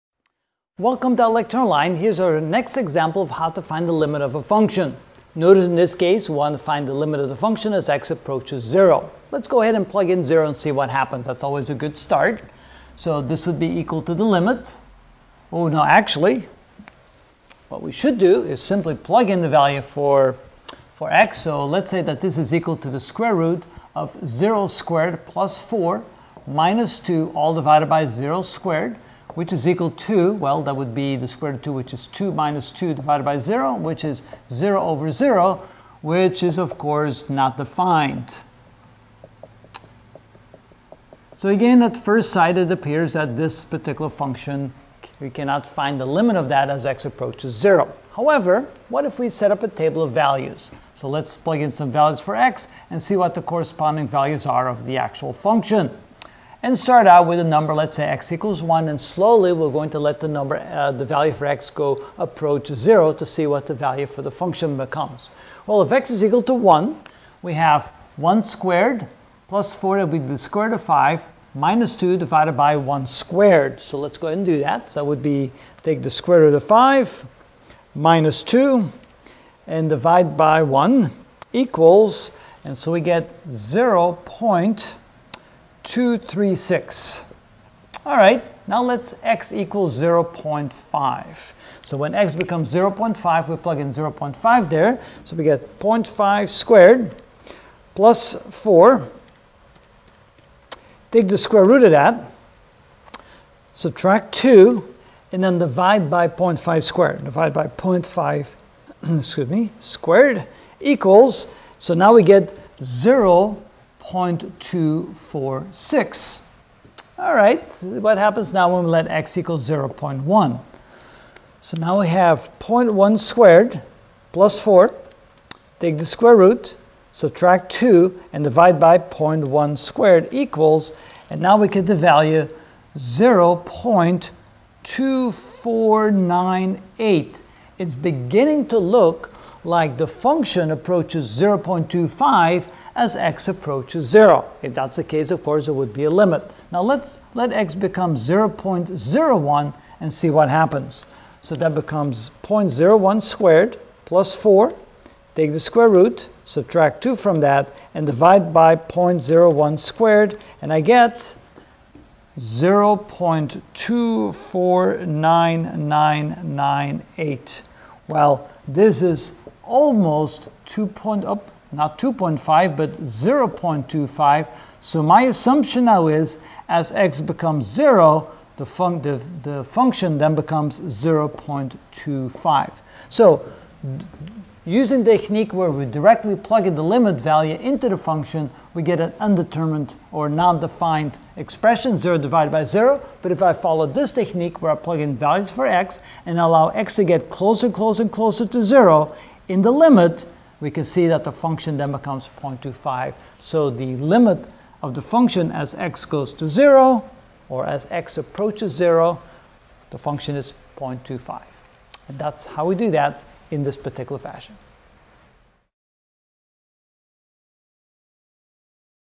Calculus 1: Limits & Derivatives (8 of 27) Finding the Limit of a Function - Example 3
In this video I will calculate the limit of a function.